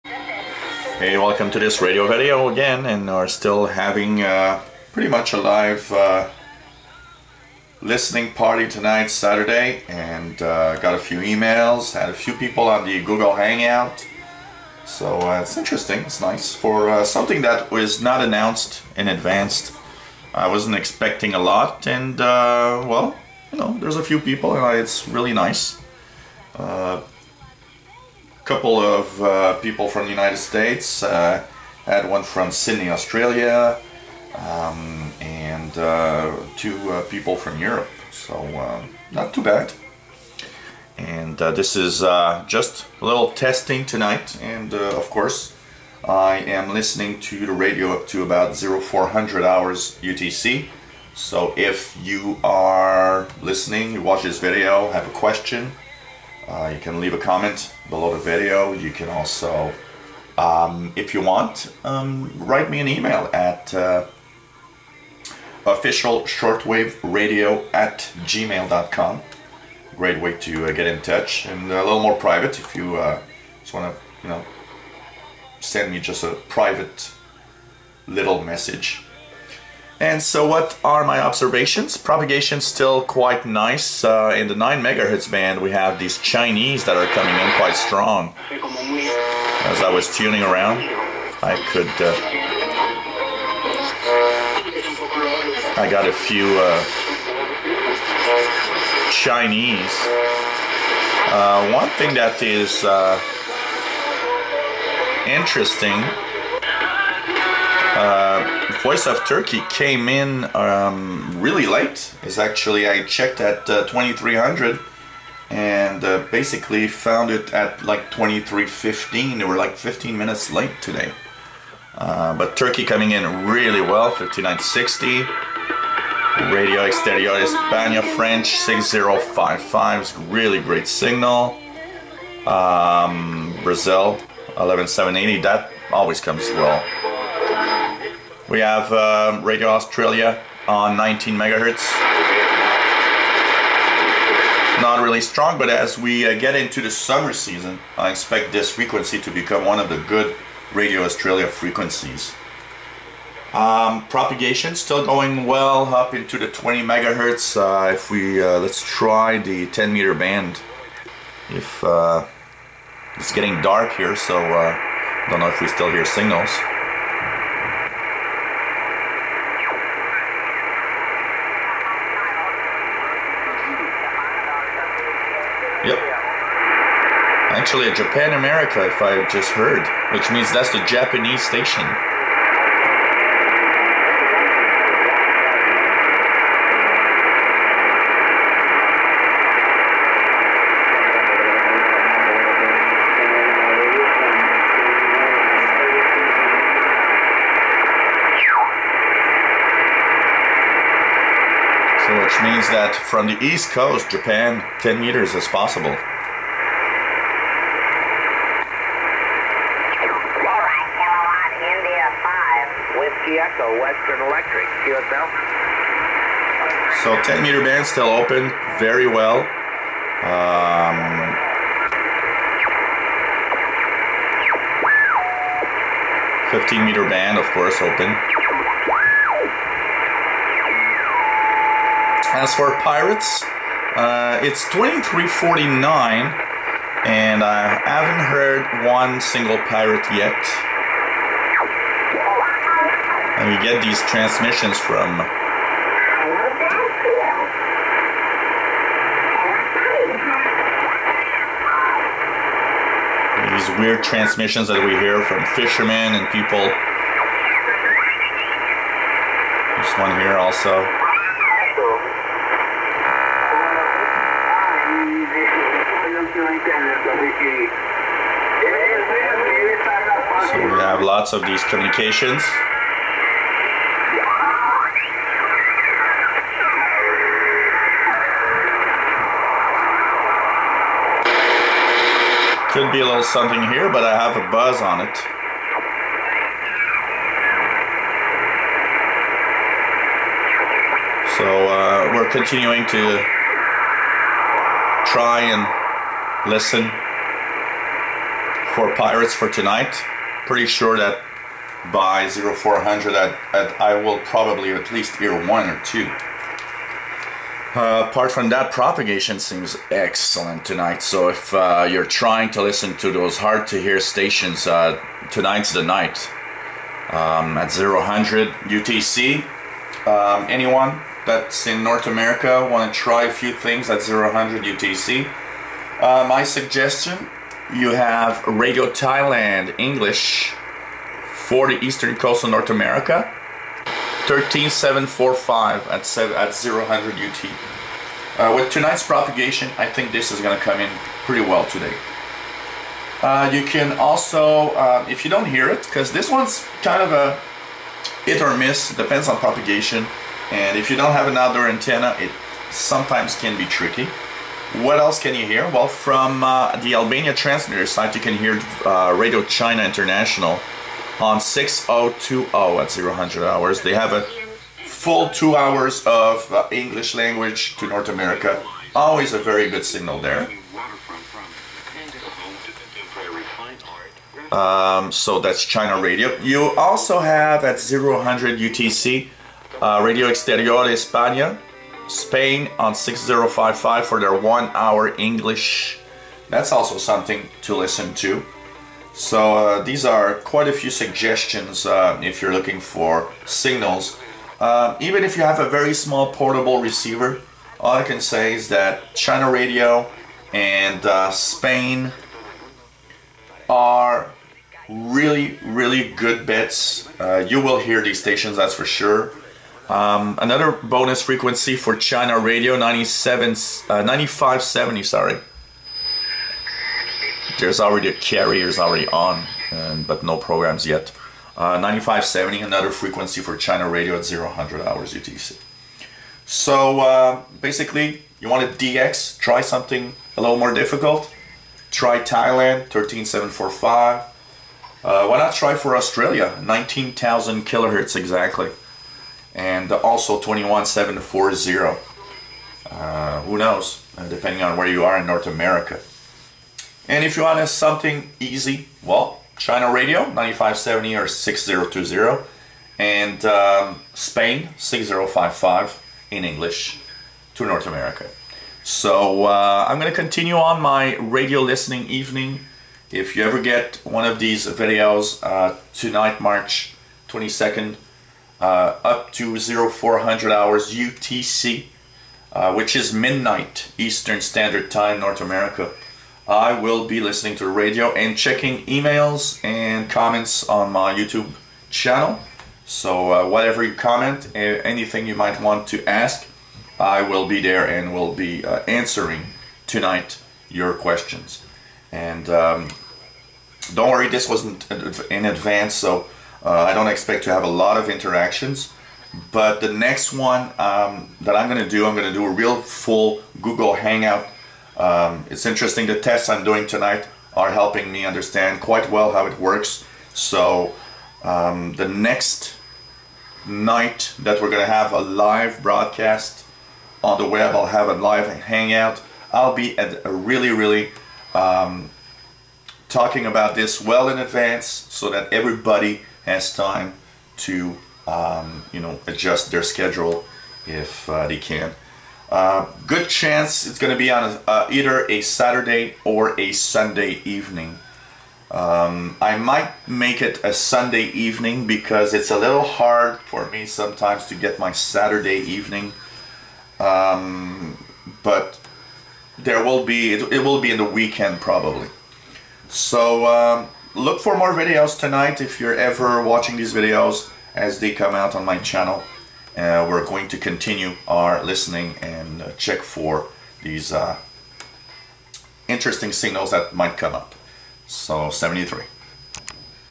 2345 UT Observations and suggestions for 0000 UT english shortwave stations
Great propagation tonight ... no pirates yet ...  many stations to look for